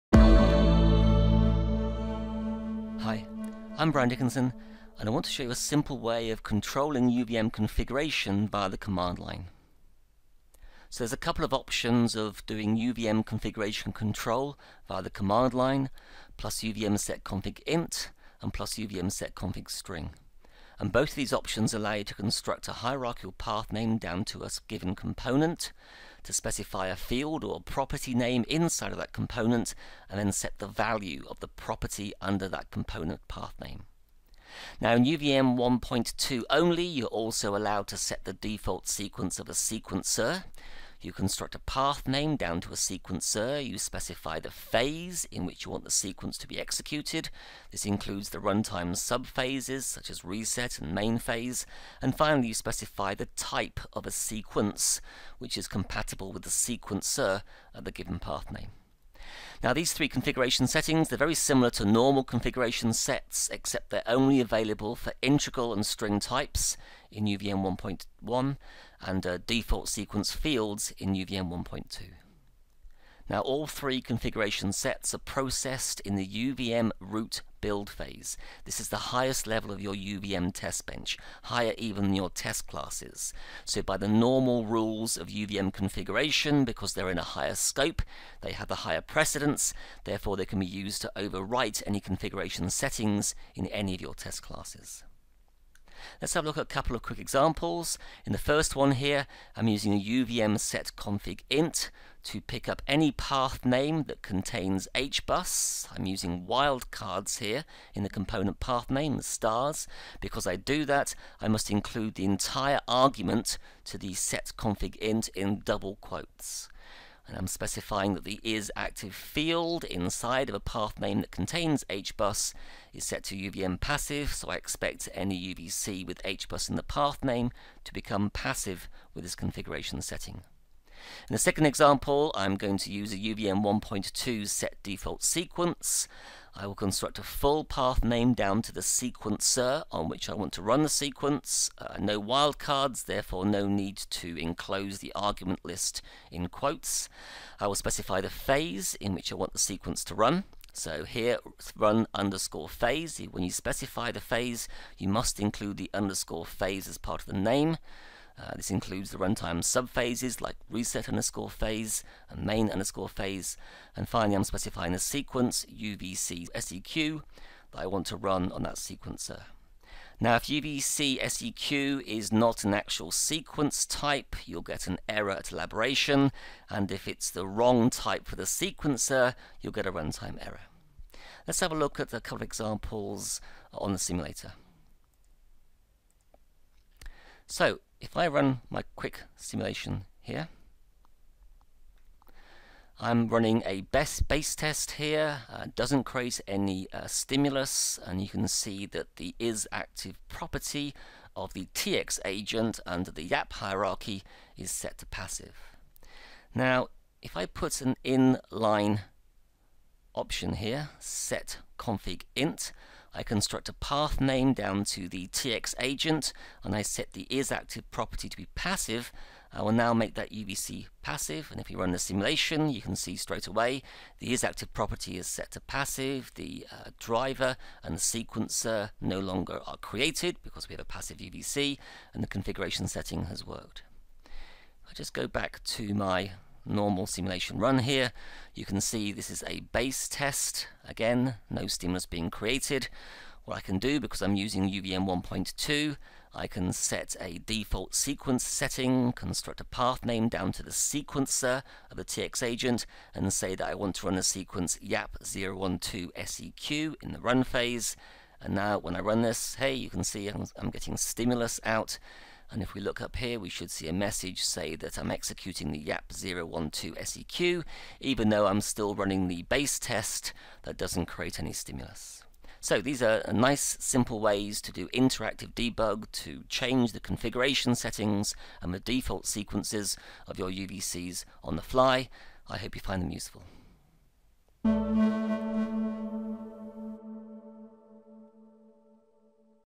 UVM Command Line Configuration Control
UVM provides simple command-line configuration control using +uvm_set_config_int and +uvm_set_config_string. Also in UVM1.2 you can set the default sequence of a sequencer using +uvm_set_default_sequence. Here we show you how to use these options for simple dynamic debug.